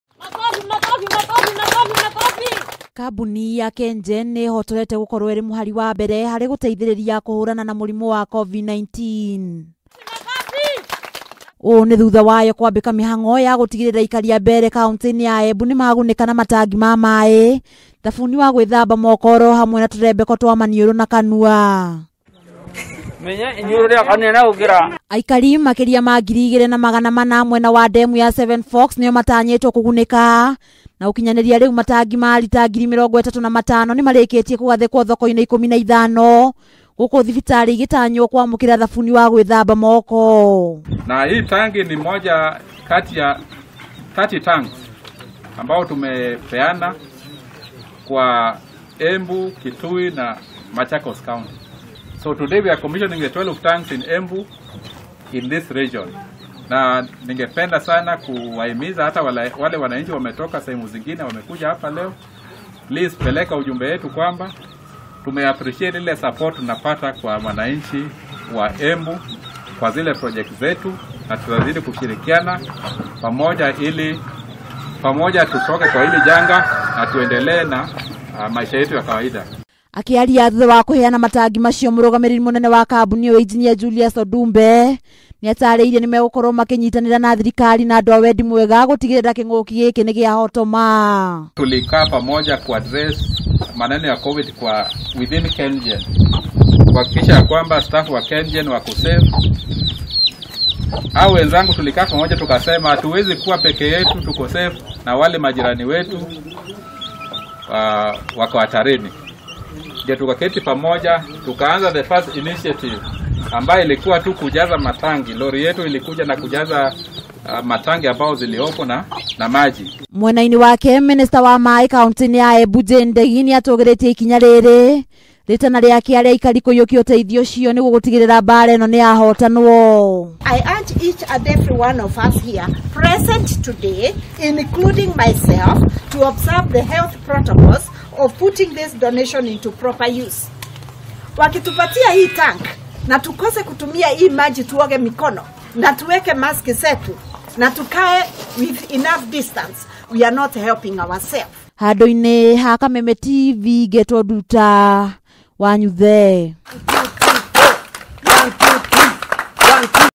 Kambuni ya KENGEN kuheana matangi Embu
Makiria ma aikari 24,000 aria maikaraga matuura-ini ma ndemu ya Seven Forks mwena wa Mbeere kaunti-ini ya Embu nimatanyitwo kugunika na matangi ma maai, maria mambiriirie kuheanwo ni kambuni ya Kengen.
Matangi mau nimabitie kugathikwo thoko-ini itiganite hamwe na thibitari itemi-ini ria kuhotomia gutherema kwa murimu wa covid 19.